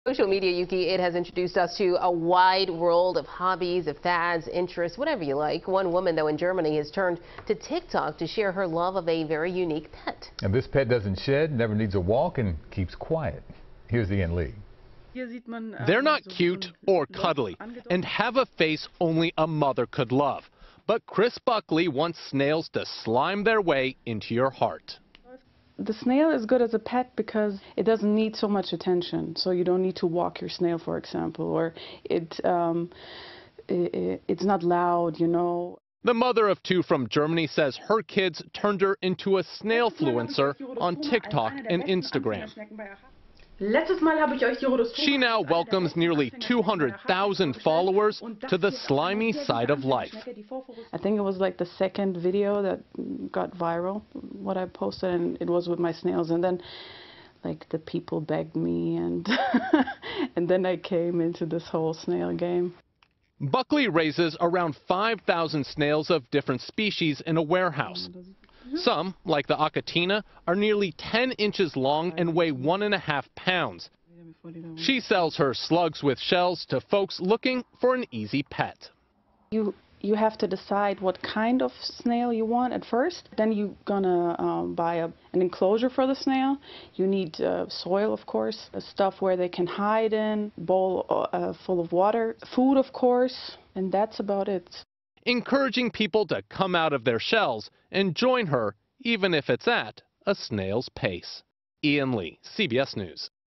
Woman shares love for snails on TikTok, why they are good pets
A woman from German is a "snail-fluencer," sharing her love for snails on Tiktok and what makes them good pets